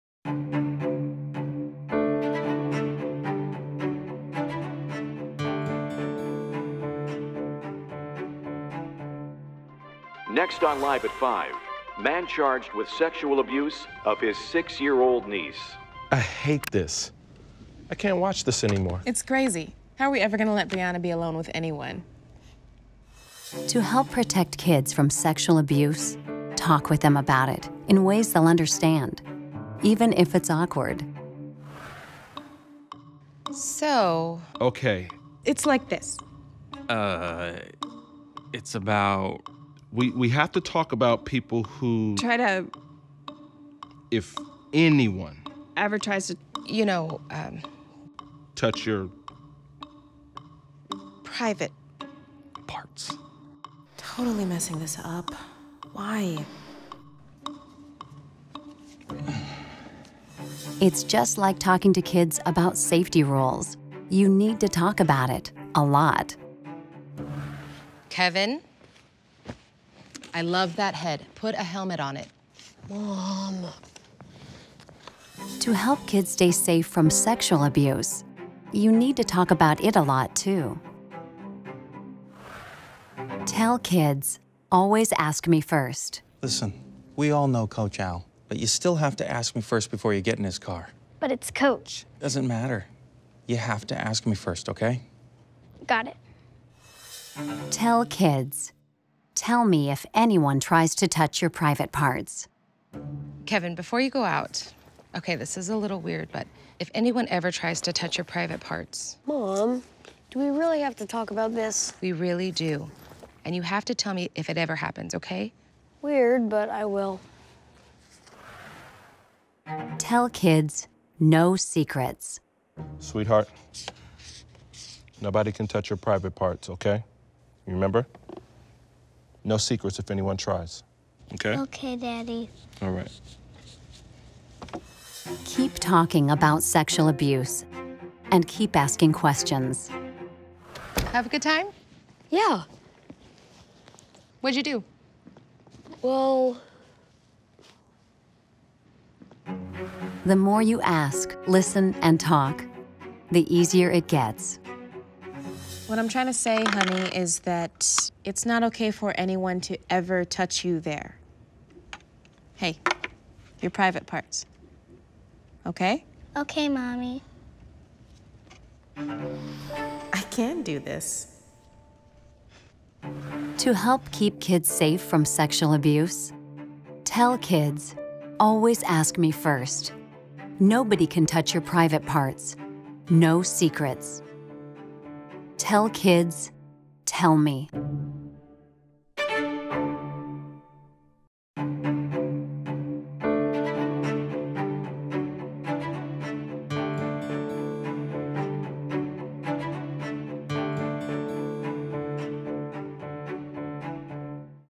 How to Talk with Kids About Sexual Abuse (Full Version)
Research shows that talking to your kids about touching safety is the best way to keep them safe from sexual abuse. Yes, it can be uncomfortable and awkward. But you can do it—just like you talk to them about avoiding drugs or wearing a helmet. This video will show you how.